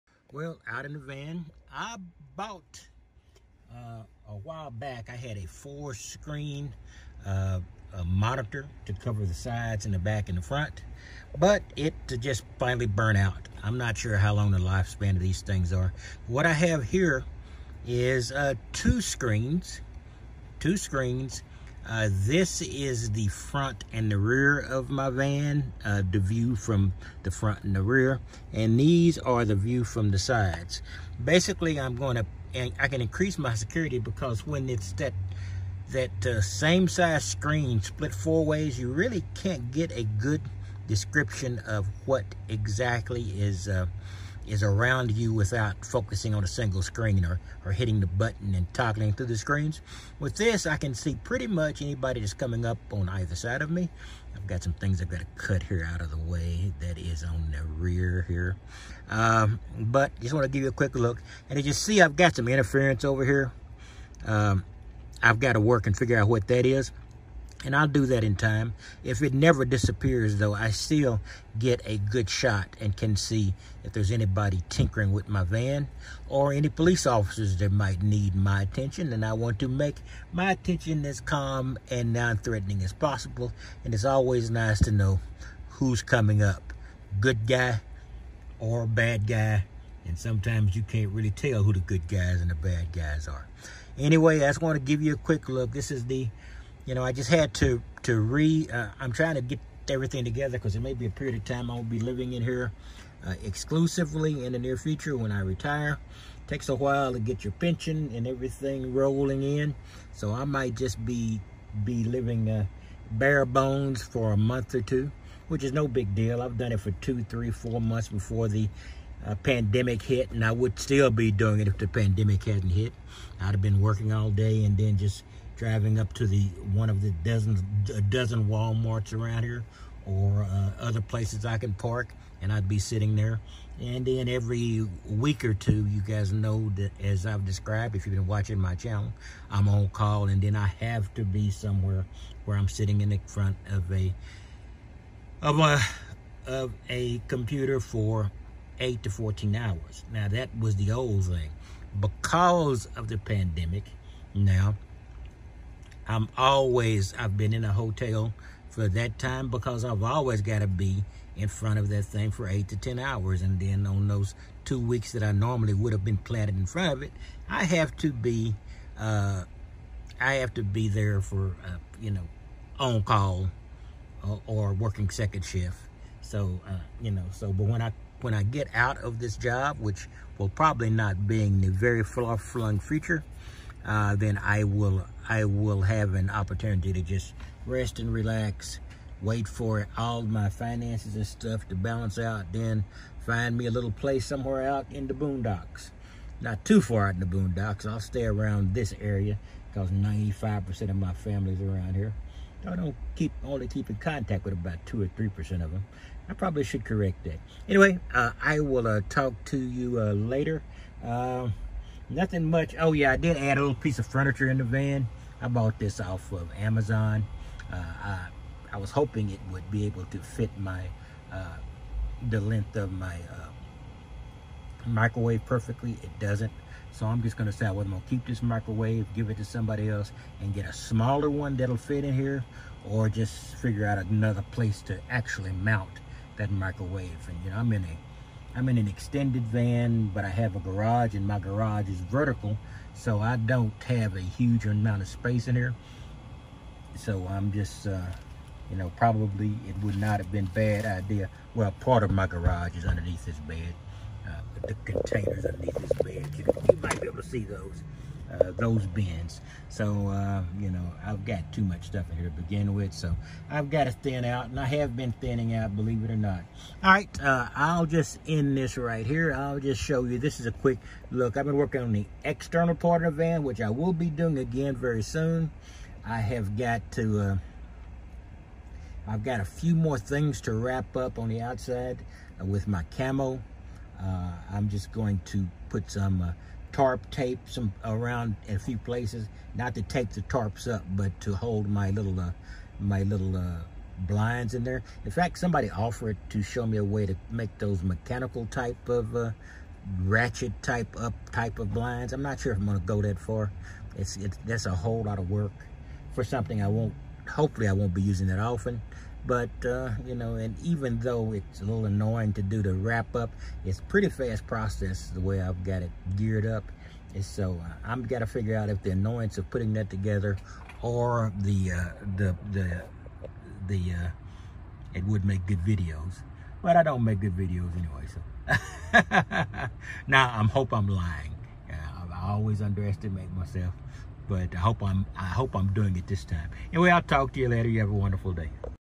Converting from an old 4 screen (backup) monitor to a dual 2 screen set up to Observe Around the Van
My old 4 camera van monitor setup died so instead I got a dual screen Monitor one for the sides and another for the front/rear facing cameras.